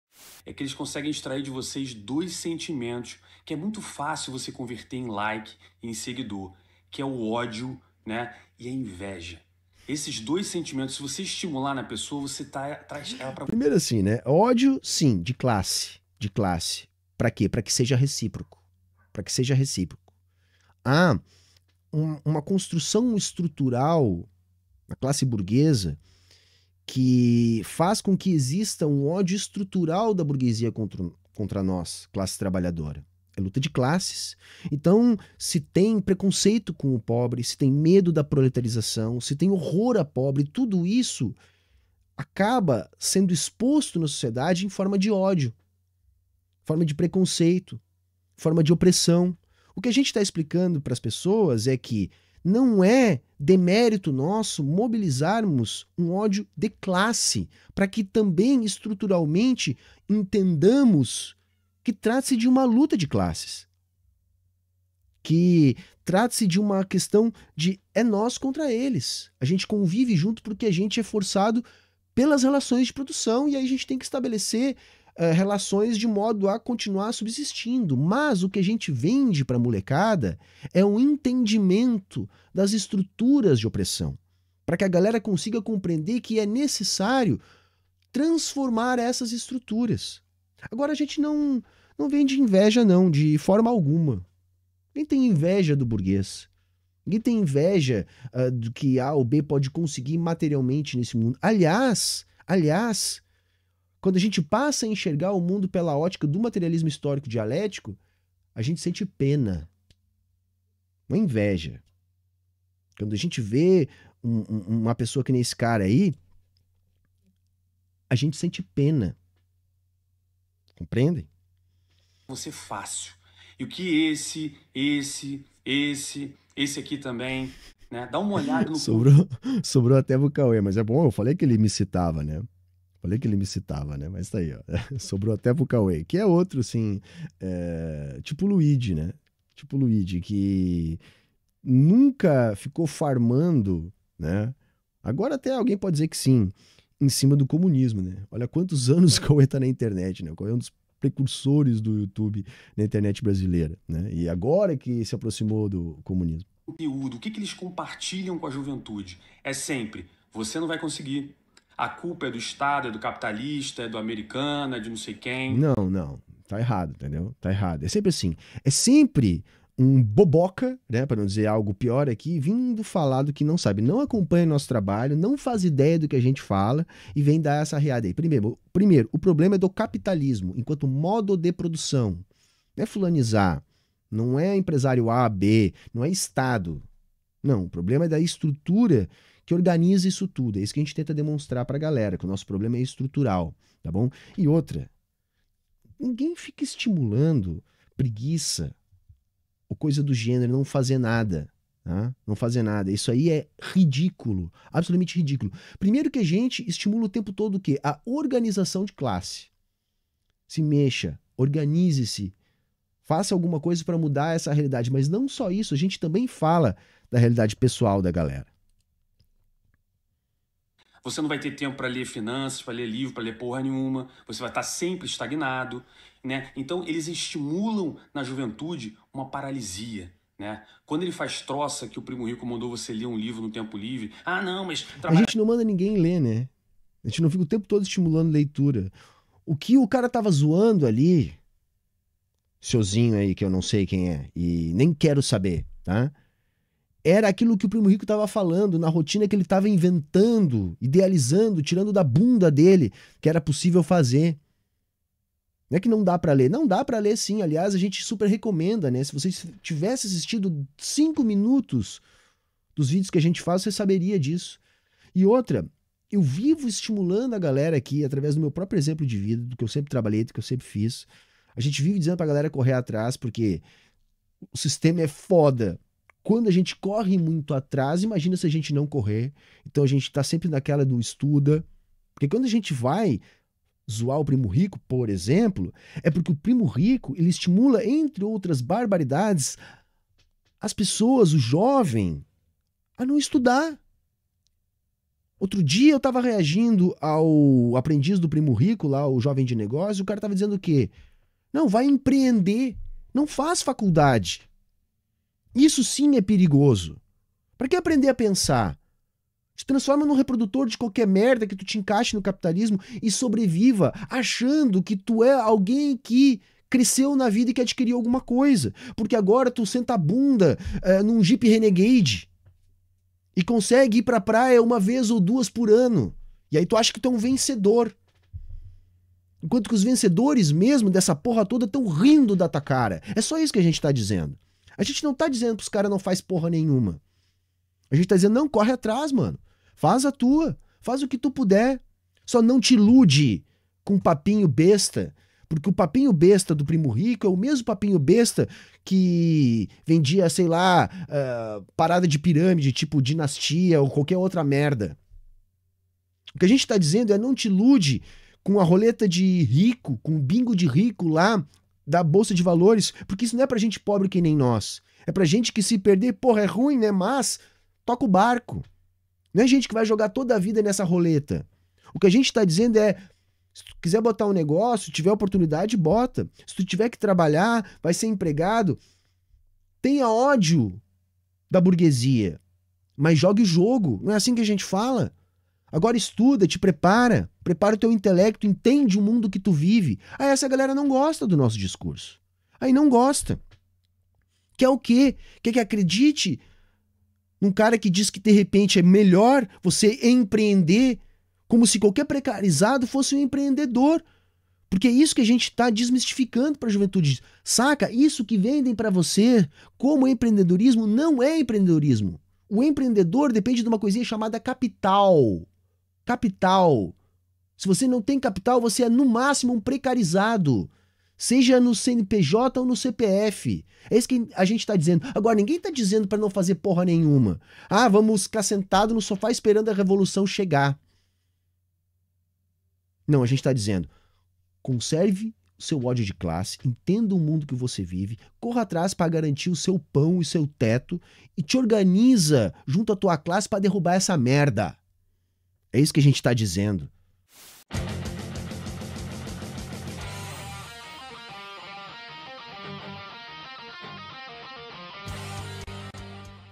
HUMBERTO ENSINA LIBERAL SOBRE TRABALHO
Corte do meu react a um liberal que me citou sem ter noção da realidade.